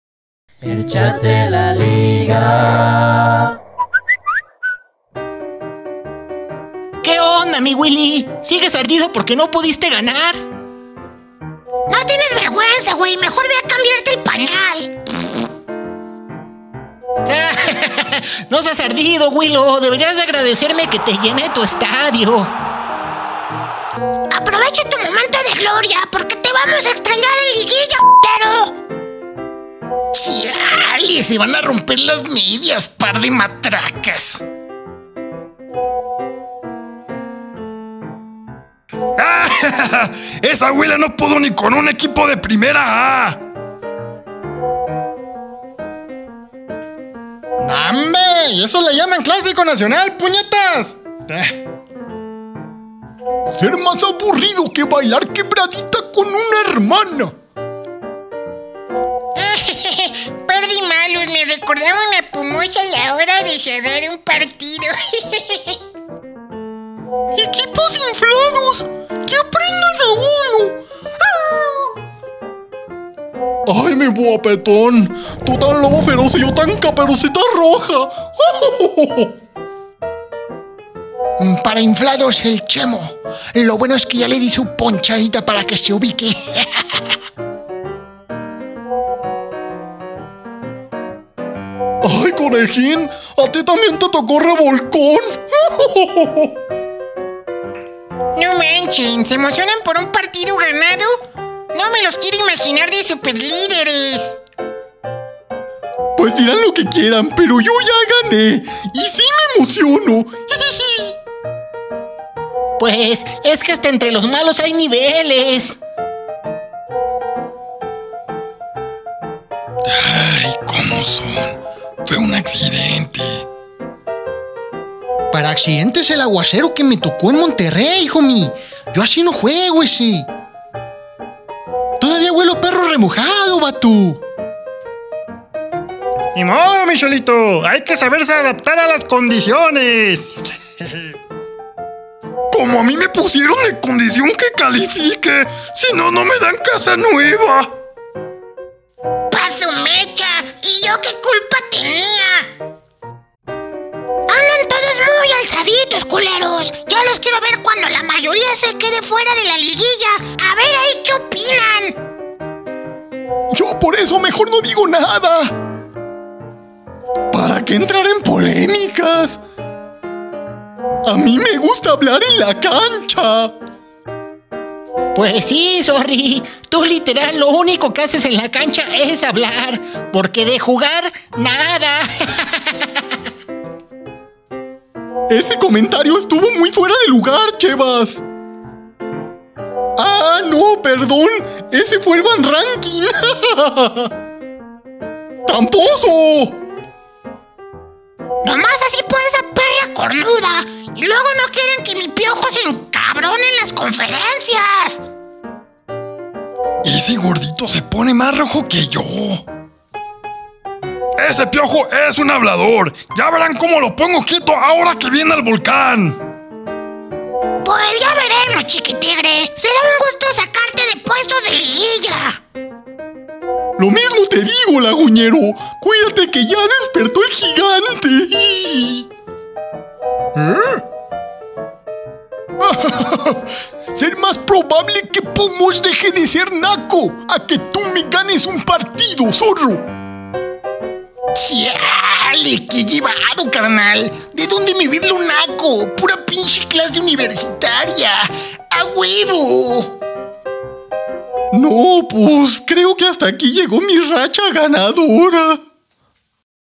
El Chat de la Liga 25 (Después del Clásico Nacional)
Ubaldo presenta

Esta semana la Carrilla no puede faltar con una jornada que hubo de todo, un clasico nacional, un triunfo de Atlas, una derrota de Cruz azul... Pfff se puso intenso 🤣😂